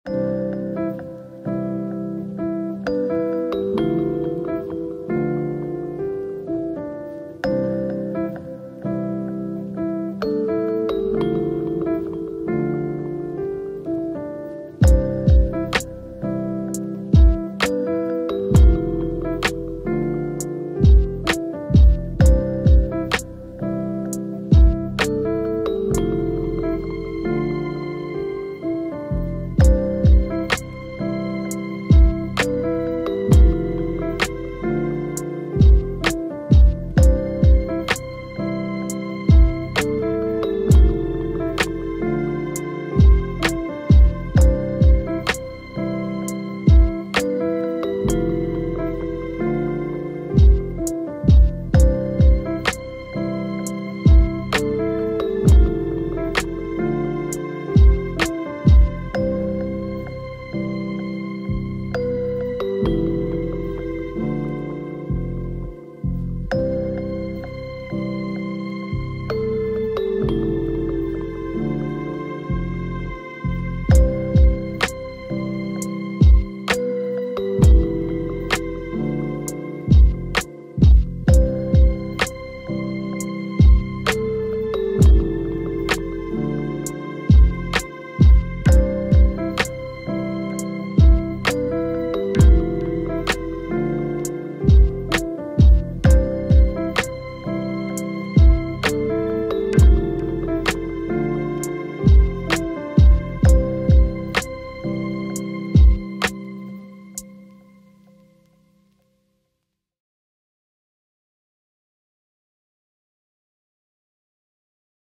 Making a Dreamy Lofi Beat with Akai MPK Mini MK2
Hi, in this video I’m using the midi controller Akai MPK Mini MK2  to create a dreamy bedroom pop chill Lofi hip hop beat.

Visit my instagram to find more:
Lofi beats, Boom bap, old school, lo-fi, jazzy beats, lo fi type beat.